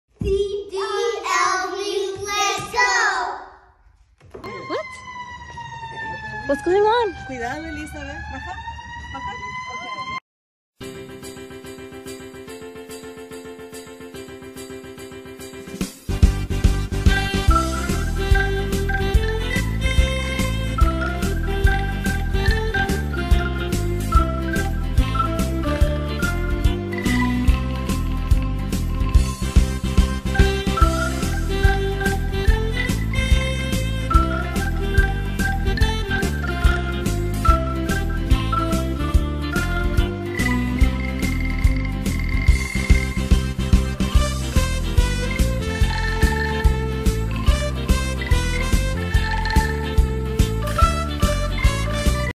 Surprise visit from Santa in a fire truck
Hold onto your stockings, because guess who made an unexpected stop at our doorstep? The man in red himself, Santa Claus, arriving in a festive fire truck! 🚒🎅✨

In this heartwarming holiday surprise, Santa has traded his sleigh for the sirens and flashing lights of a fire truck, making a special visit to spread Christmas joy. The excitement is contagious , and the air is filled with the sound of jingle bells and cheerful laughter.

Watch as Santa steps down from the fire truck, carrying a bag full of surprises and a twinkle in his eye. It's a unforgettable moment filled with wonder and delight as he greets each member of the family, leaving behind smiles, warmth, and memories to last a lifetime.